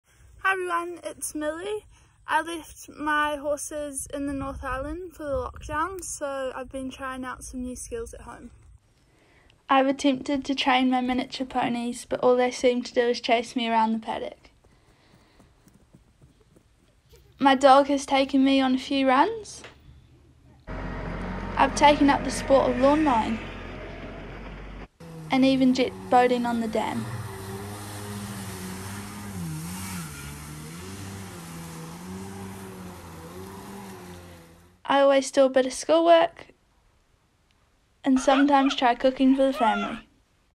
Millie's iso video
Head of Equestrian, Millie, may have had to leave her horses in the North Island for lockdown, but she's kept herself very busy lawnmowing, trying to train miniature ponies and even jet boating on the dam!